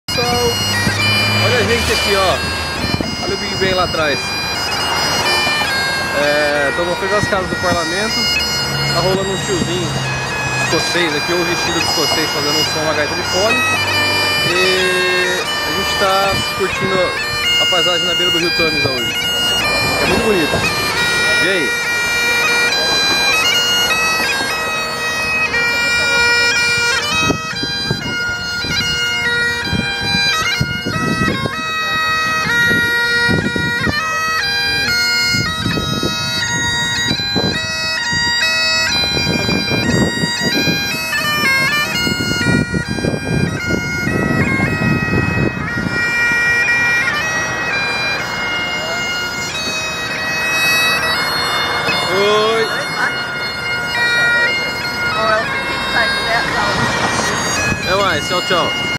Gaita de Fole em frente ao Big Ben
Apresentacao de artista de rua escoces (ou nao!) tocando gaita de fole em frente ao Parlamento Britanico